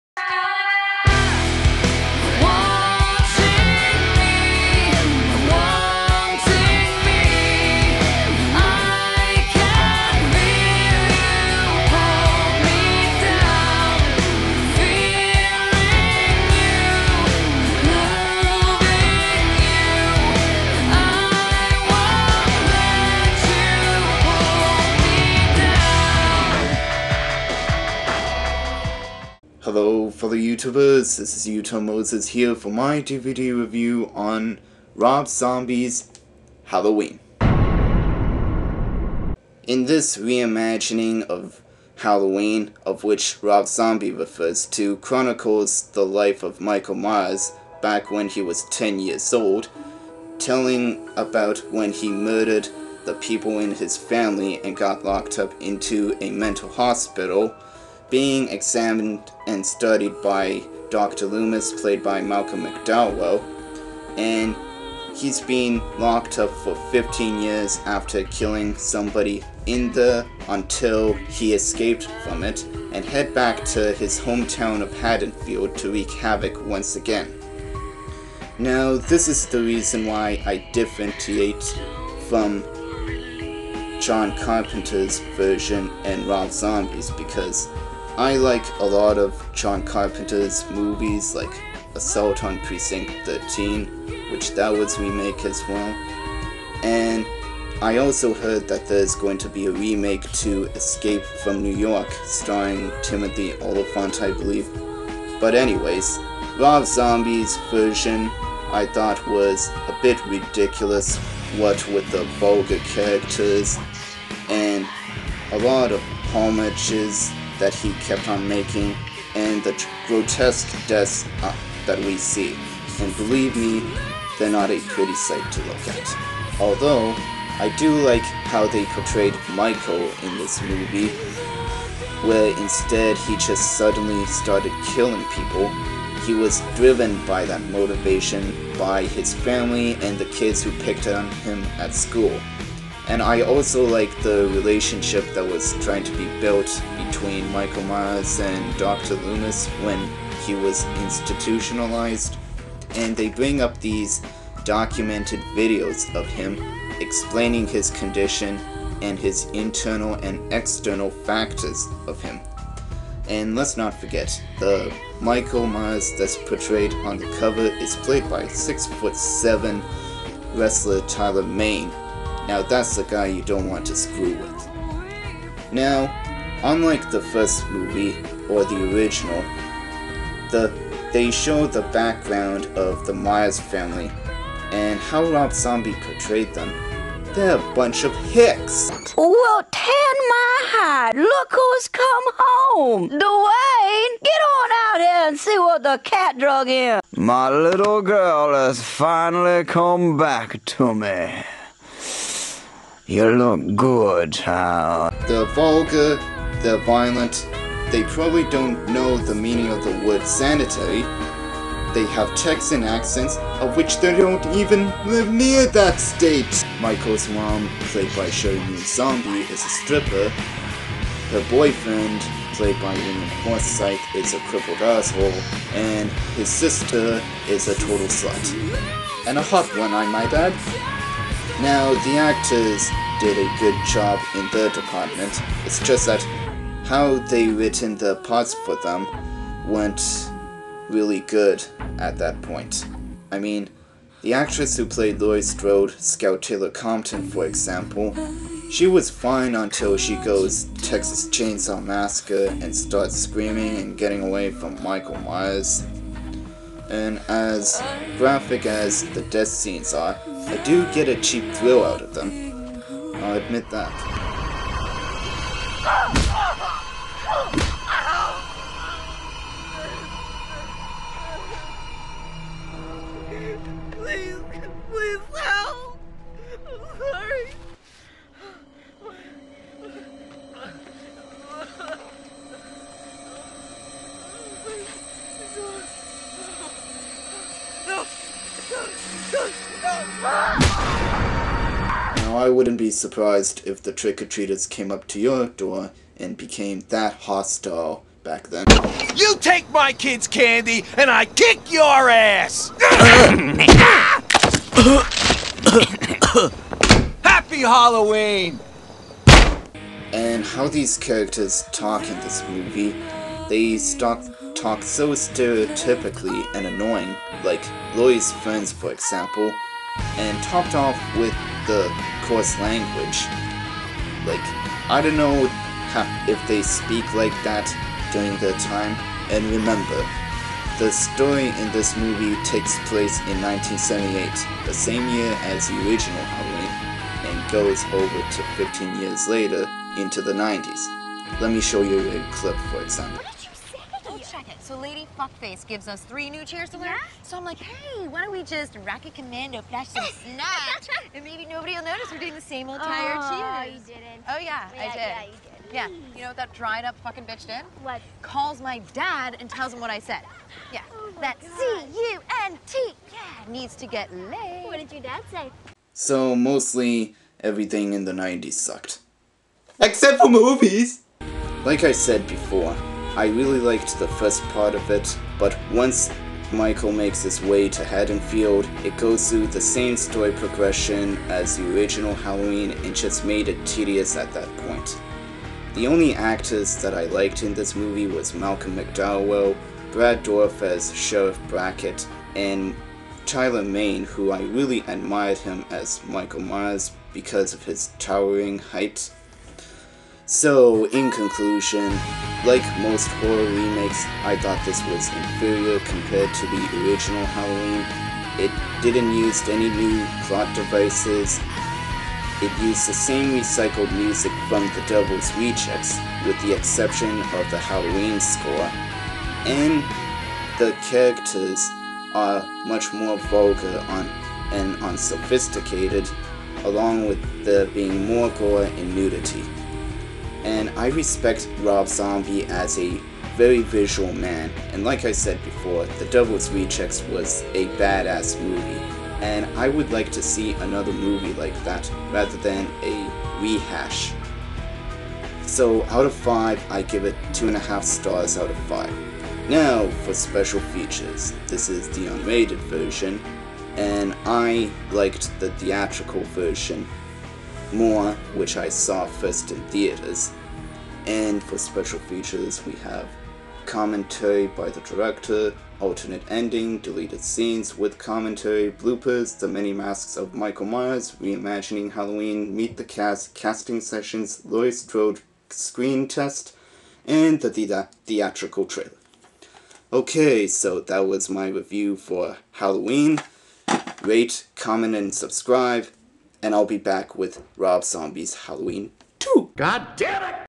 Rob Zombie's Halloween DVD Review
Here's my review for the dreaded reimagining of Rob Zombie's Halloween. I've done this review on the 20th of October. Hope you enjoy